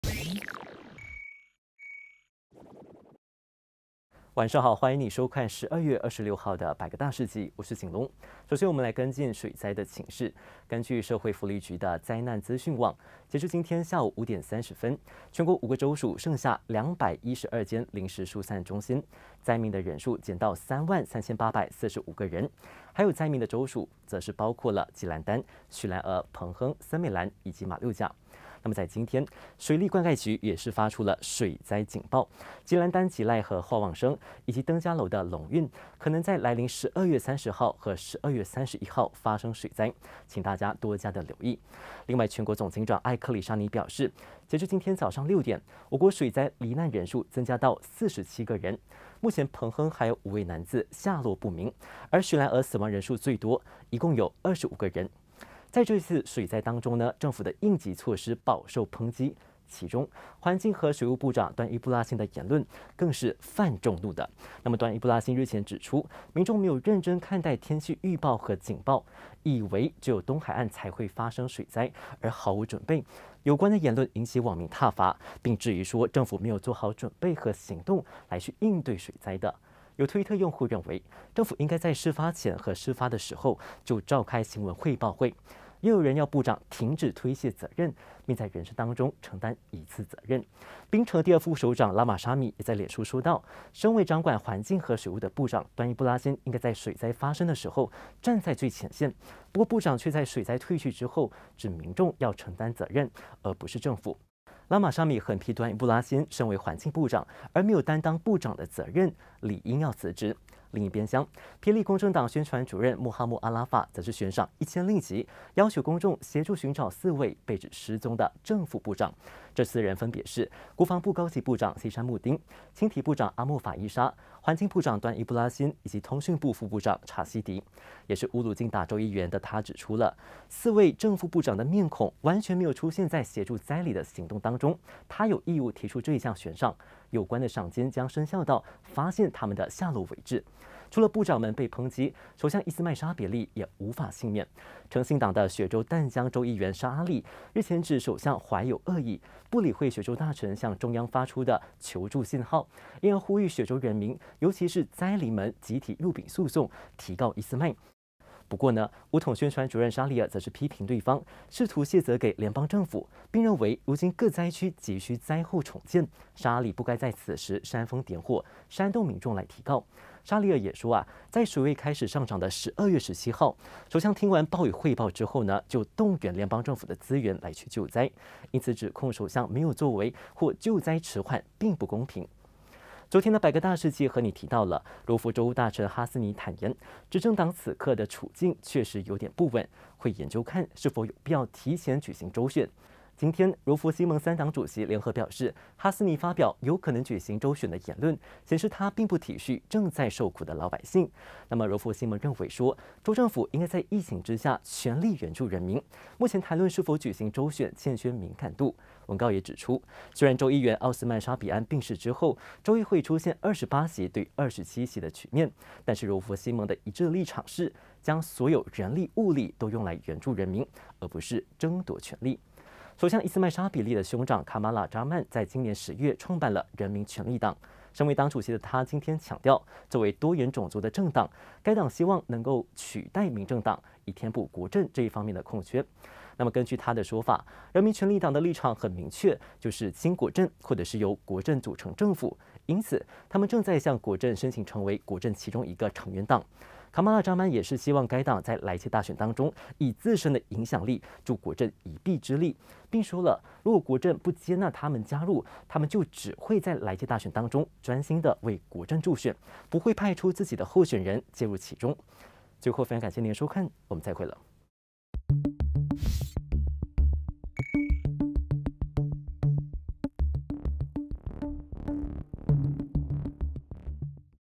百格大事纪｜柔佛提前州选？柔希盟：现时应全力援助人民
柔佛会否闪电州选，备受瞩目。今日柔佛希盟发联合文告表明立场，认为州政府应该在疫情下全力援助人民， 目前谈论是否举行州选欠缺敏感度。

12月26日 百格大事纪 一起关注各焦点新闻

【其他重点新闻】

·47人命丧水劫 水利局发丹登水灾警报

·环境部长话犯众怒 拉玛沙米促辞职

·4正副部长“失踪”？ 霹议员悬赏1千

·巫统领袖为首相护航 抨议员“煽风点火”
·柔佛提前州选？柔希盟轰：欠敏感度

·权力党盼取代“民政” 填国阵空缺
依斯迈沙比利